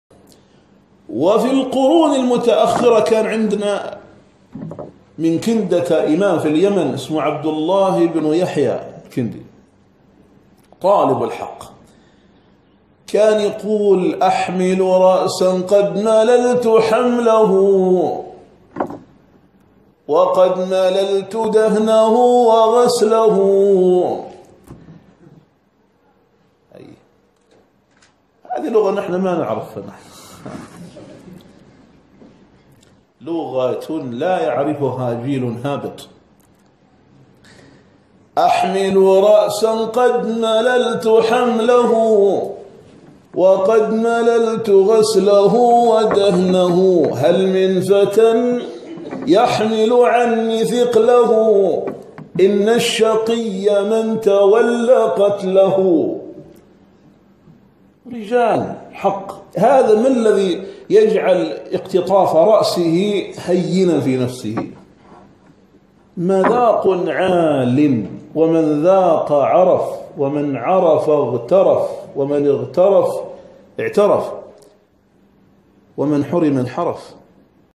شعر يَحملُك ويحلق بك الى الجنة. الشيخ مسعود بن محمد المقبالي
مصدر المقطع:

محاضرة الشيخ مسعود بن محمد المقبالي

في الكلية التقنية بإبراء